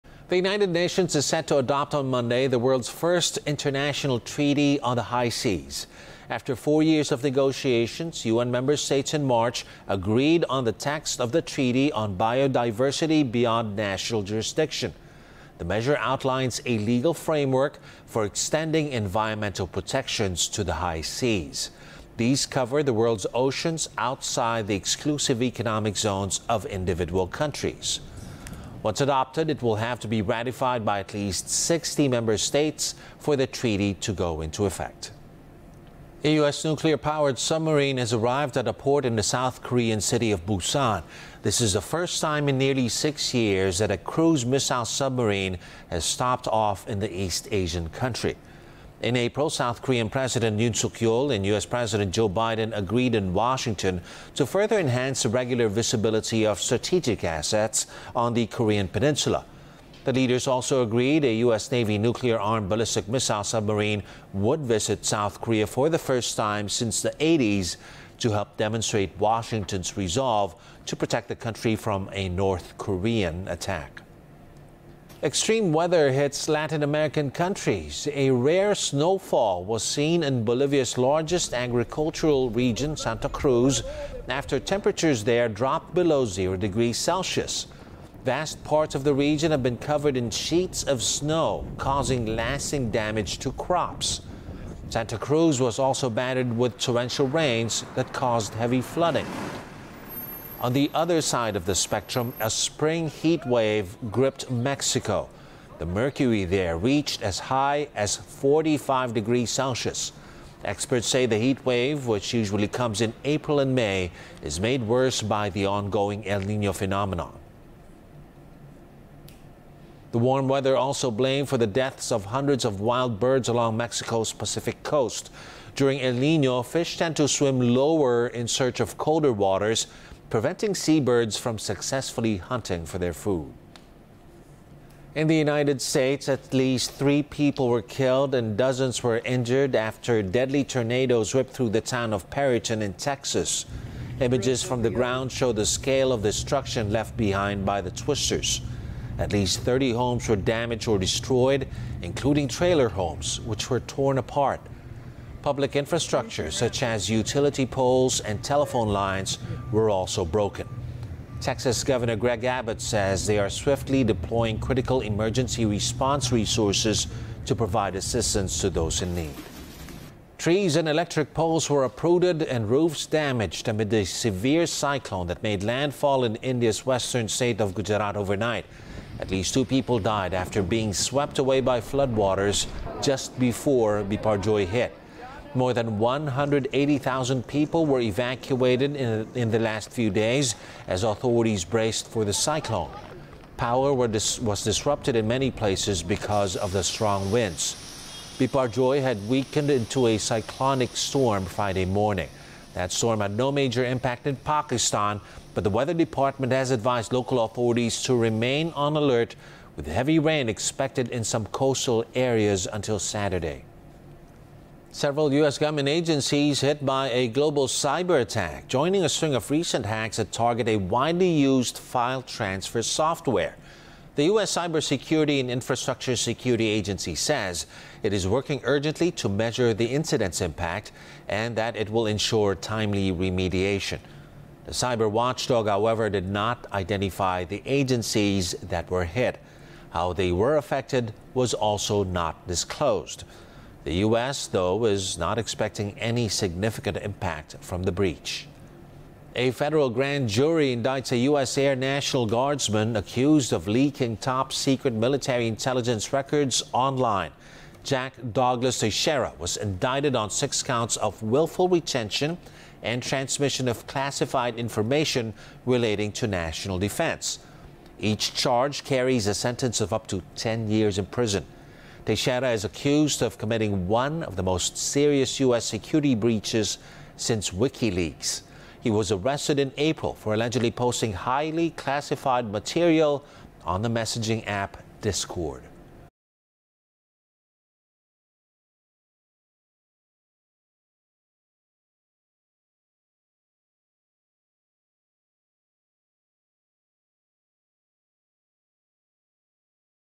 UN to adopt treaty on high seas protection | ANC
Rundown: The United Nations is set to adopt on Monday, the world's first international treaty on the high seas.